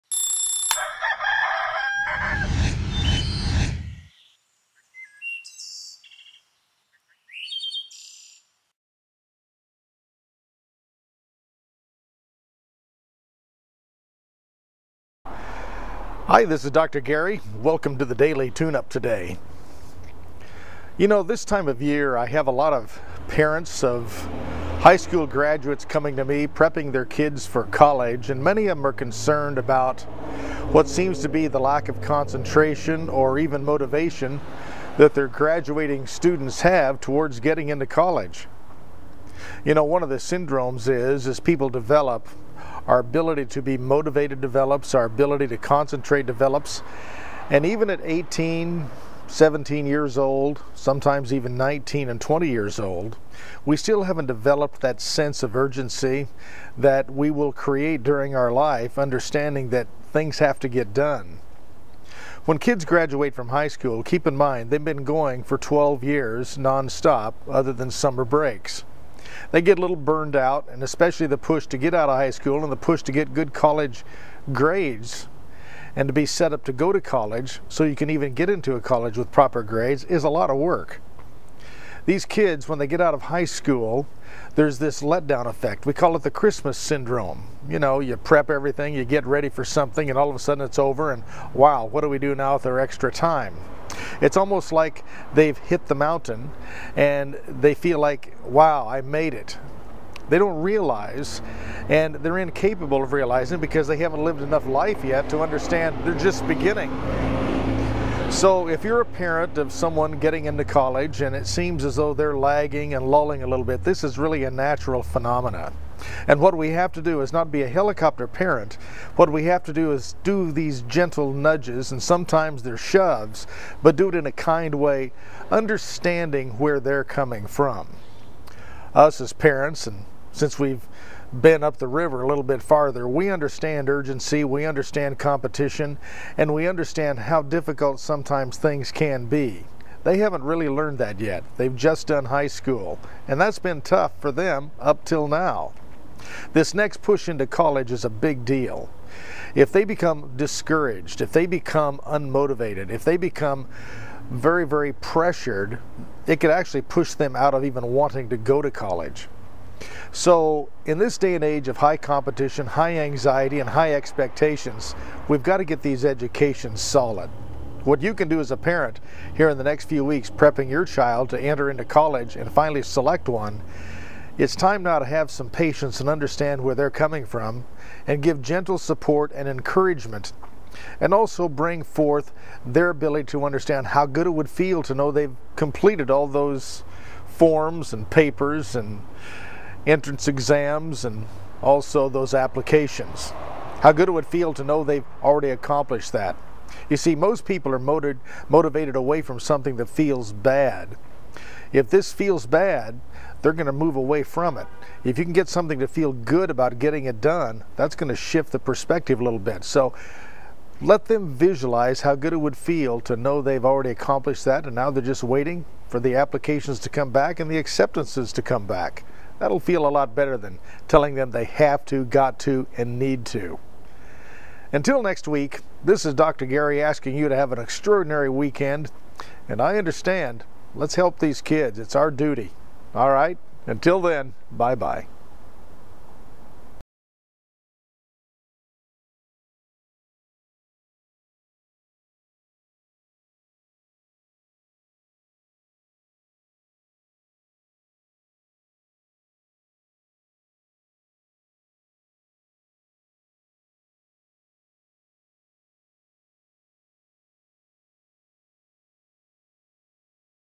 Having Challenges with College Prep
Here is a tip on getting your procrastinating student ready for college